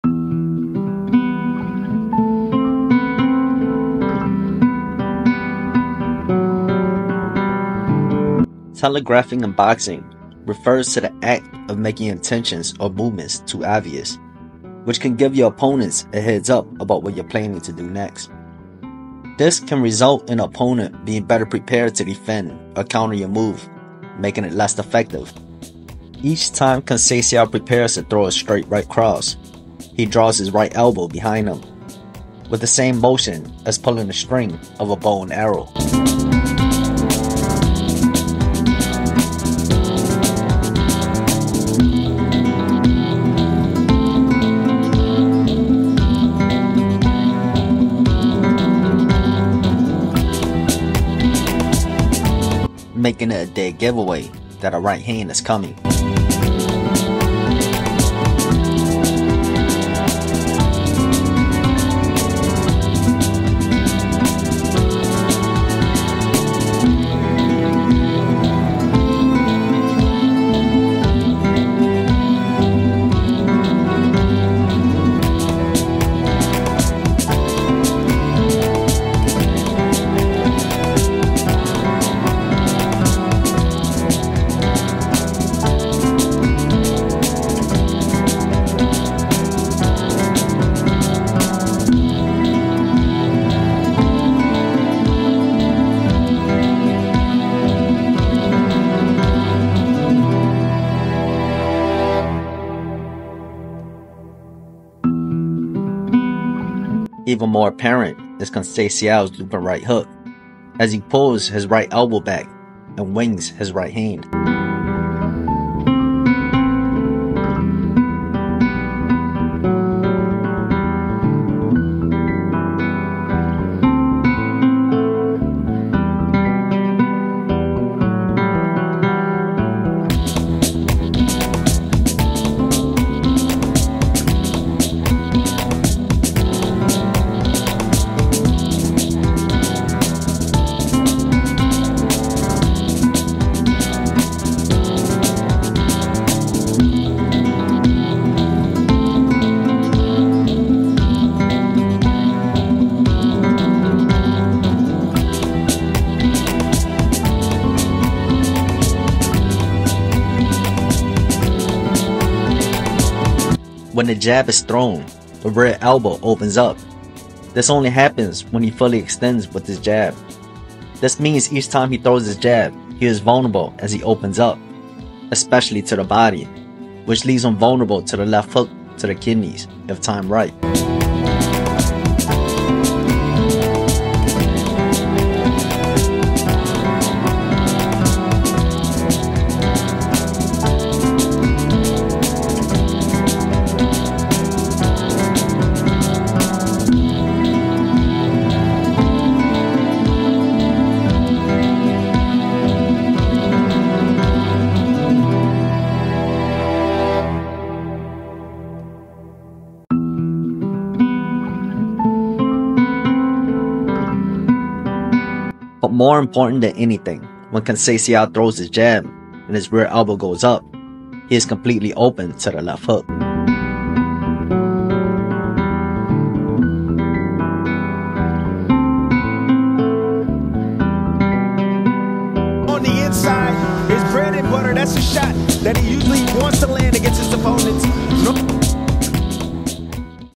Robson Conceicao | BAD HABITS | Reading & Countering Telegraphed Punches
Conceicao vs. Foster II is around the corner. The first fight was a very controversial decision. Foster must make sure he leaves no doubt in the judges mind & take advantage of Conceicao’s bad habits.

00:00 Telegraphing
01:43 Looping Punches
02:39 Opening Up